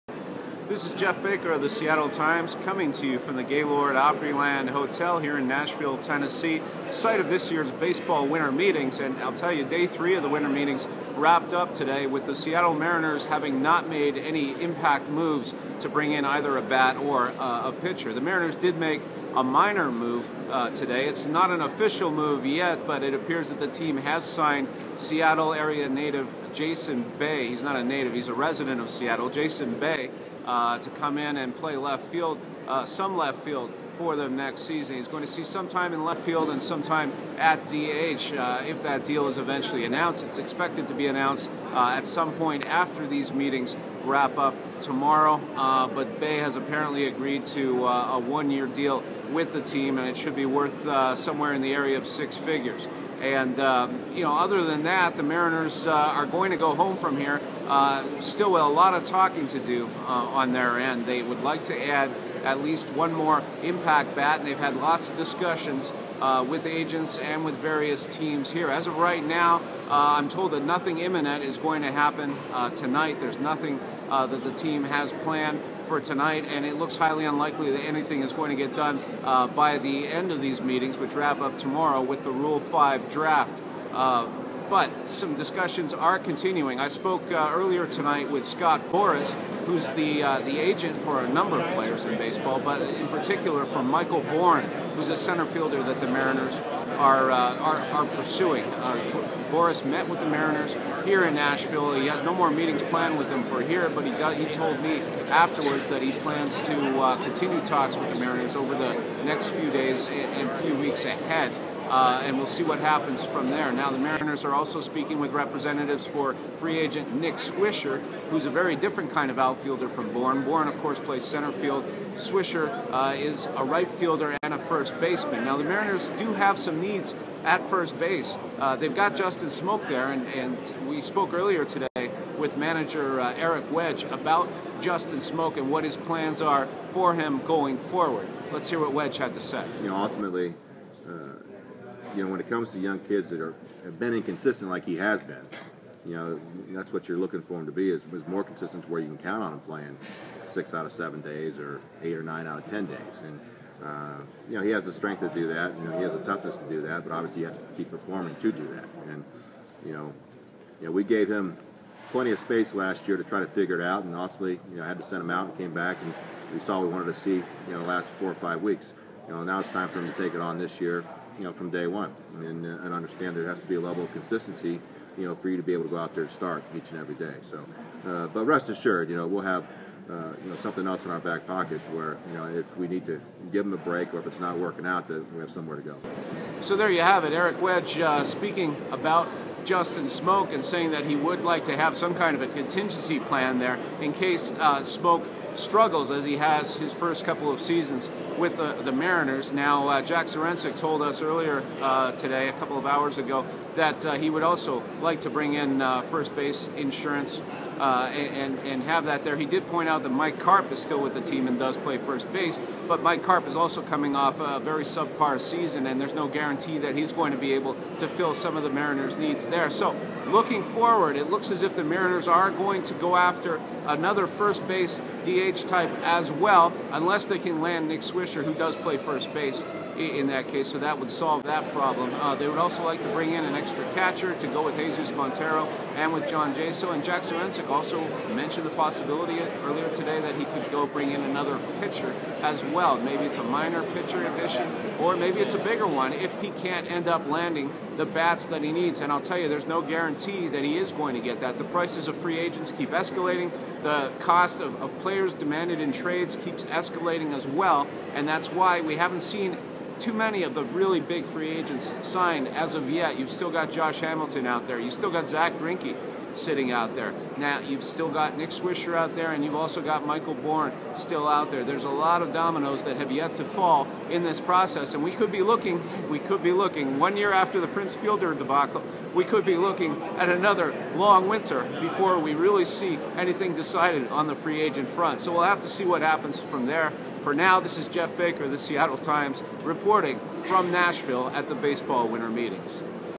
Wrapping up Day 3 at baseball winter meetings
Seattle Times baseball writer Geoff Baker wraps up Day 3 at the baseball winter meetings in Nashville.